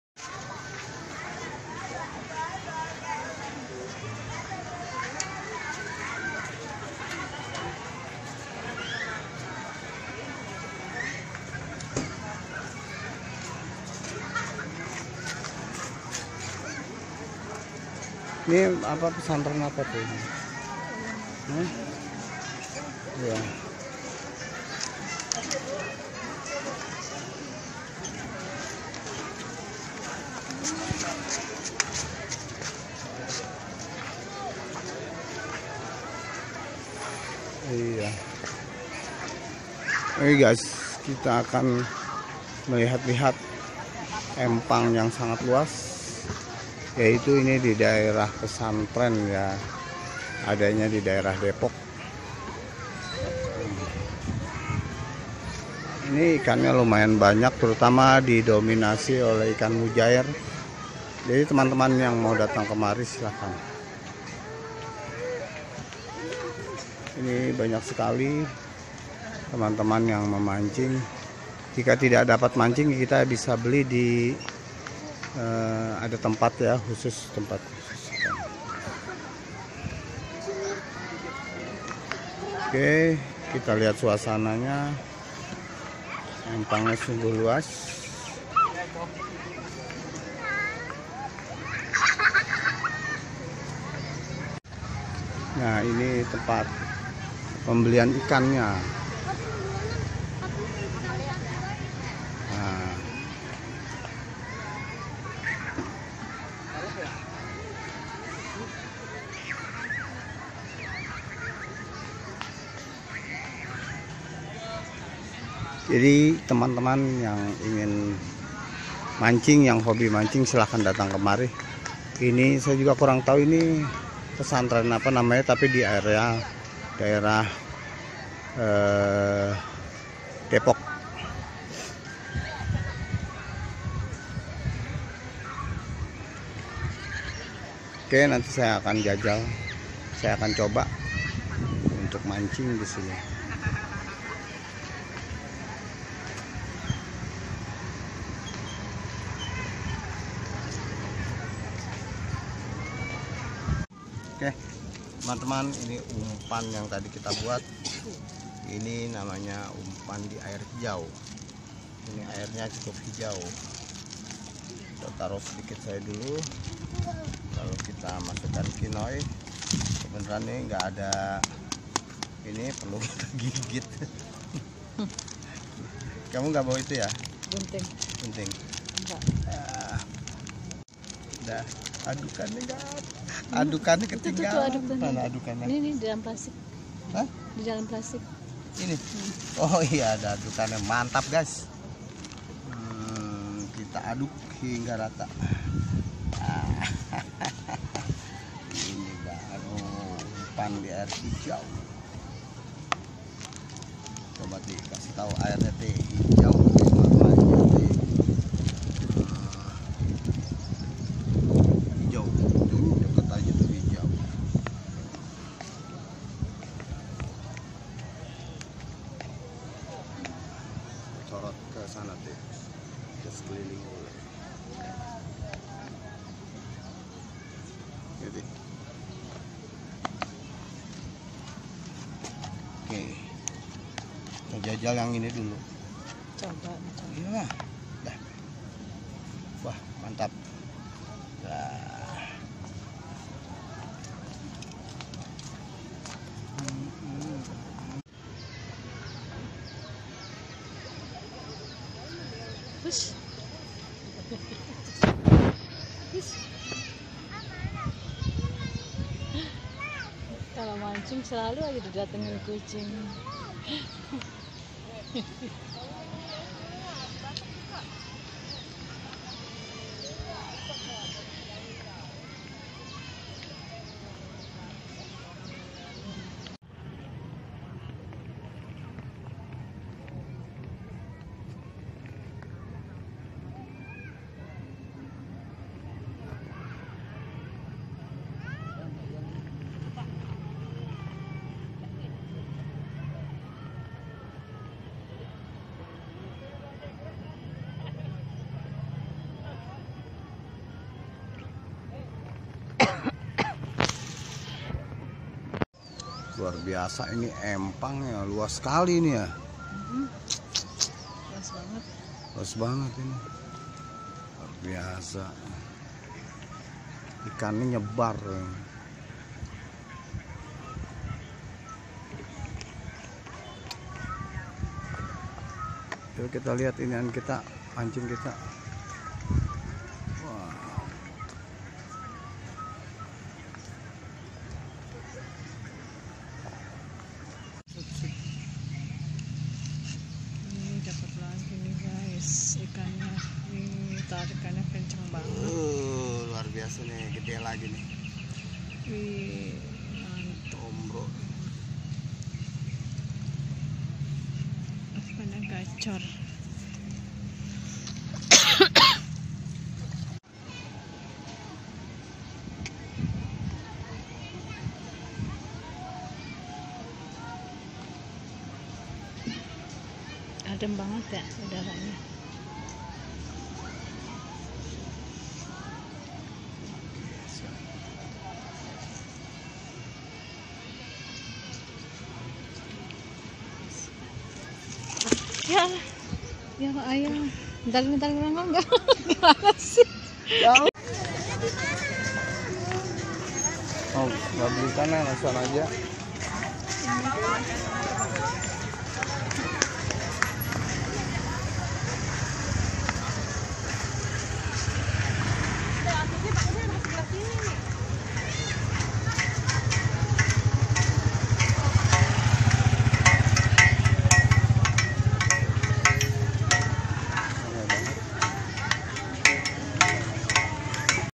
Empang Luas Di Dalam Pesantren Di Depok Jawa Barat
mencoba umpan ikan mas dan bawal serta Empangnya sangat luas dan didalam pesantern besar dan sangat luas, tempat refresing melepas lelah selama 5 hari kerja sungguh sangat mengasikkan hingga tak terasa hari menjelang sore